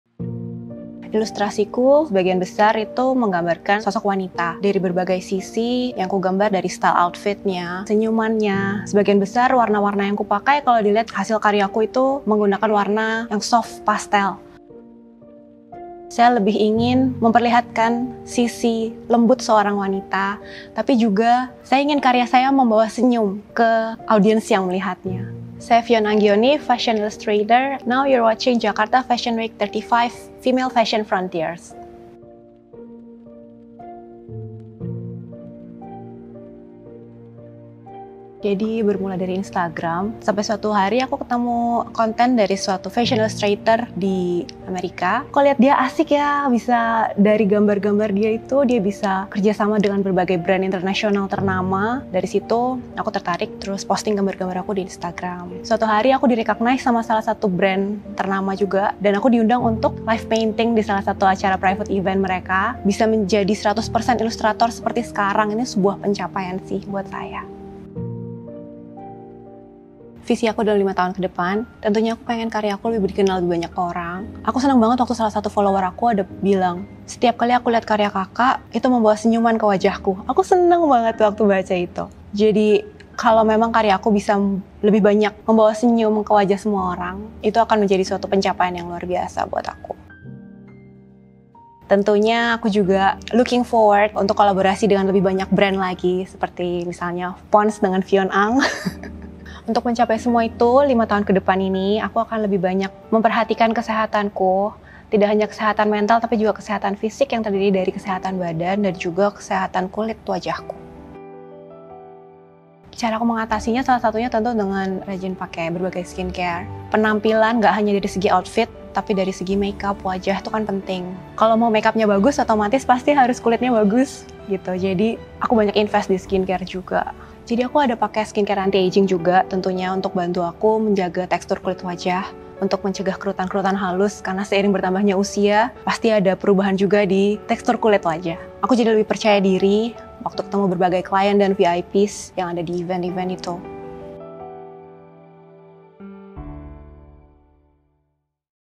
35 Female Fashion Frontiers with Fion Anggioni: Jakarta Fashion Week x POND’S Age Miracle
Fashion illustrator Fion Anggioni, which is recognized by her illustrations works for several luxury and high fashion brands shares her journey on pursuing career on the fashion industry in the new series “35 Female Fashion Frontiers” by Jakarta Fashion Week x POND’S Age Miracle.

35 Female Fashion Frontiers shows an intimate portrayal about 35 dynamic women, youthful pioneers, all 35 and under, in her journey throughout their career and womanhood to NeverStopGrowing. From models to photographers, designers to stylists, and illustrators, each weaves their uniques essence to NeverStopGlowing.

Like a spellbinding chapter in their journeys, the MiracleHappens. The culmination of their tireless efforts, reminding us all that behind every women’s journey lies the miracle of her hard work and unyielding determination.

Only on JAKARTA FASHION WEEK Youtube channel

Director: Zachary Ibrahim
Interviewer: Carra Nethania & Firyal Shabirah
Photographer: Gayuh Prasongko
Production House: Biro Baur for JAKARTA FASHION WEEK